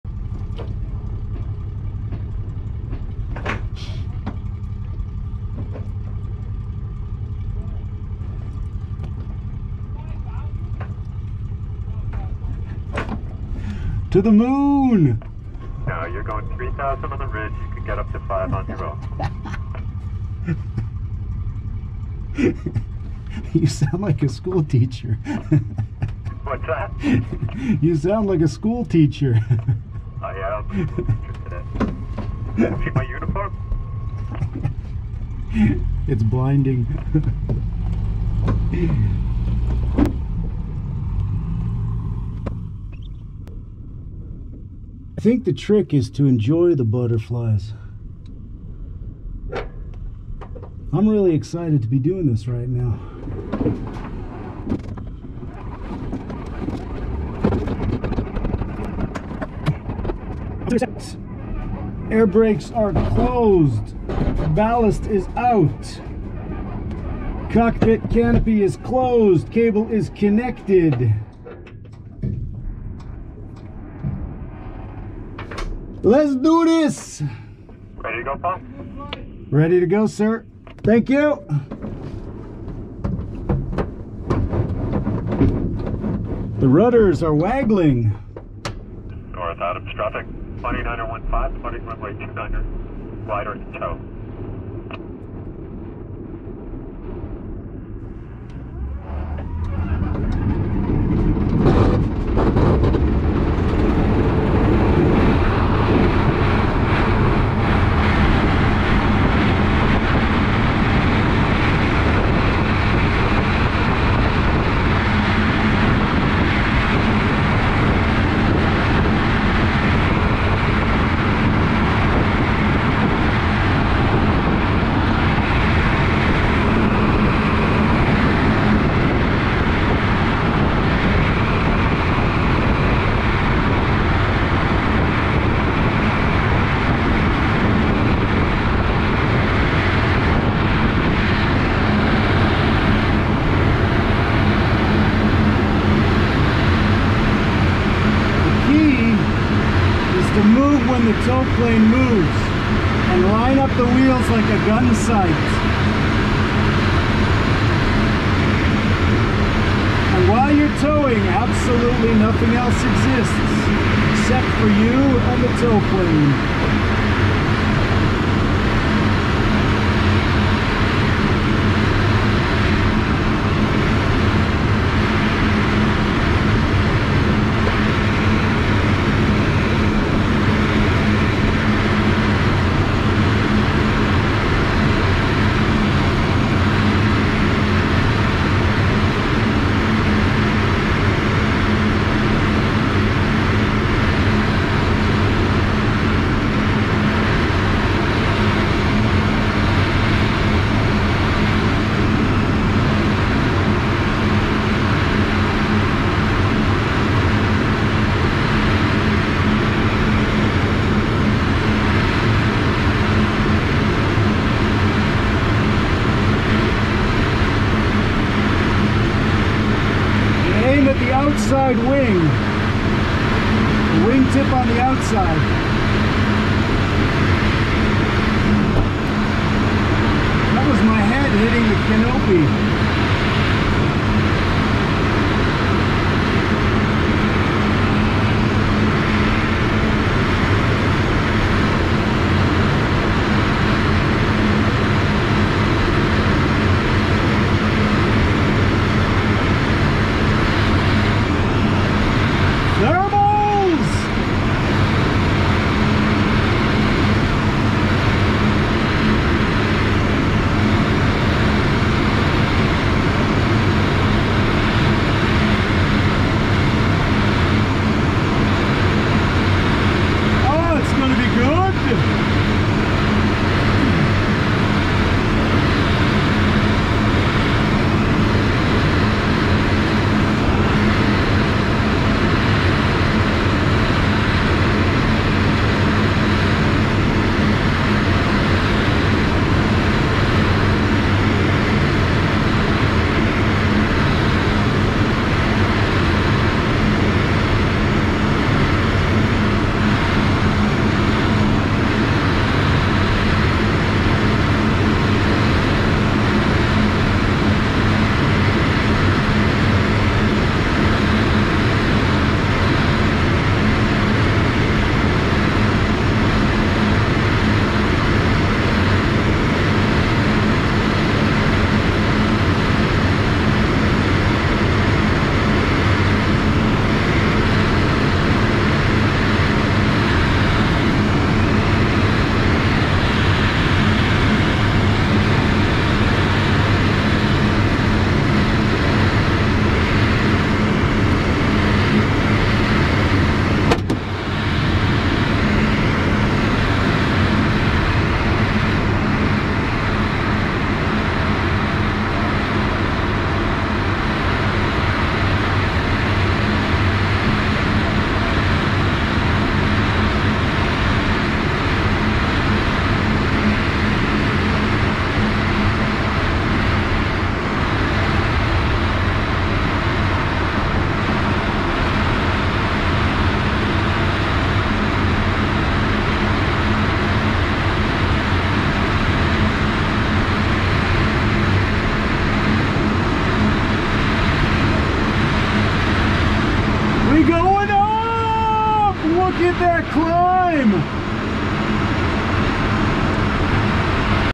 Tow to 3000 feet at KAQW
A video I wish I had when I was new to gliding.  Learning to tow smoothly in a sailplane is challenging. It's essentially flying in formation two hundred feet behind an airplane. If you know any aspiring glider pilots, please do share this with them. The tow Plane is a piper Pawnee.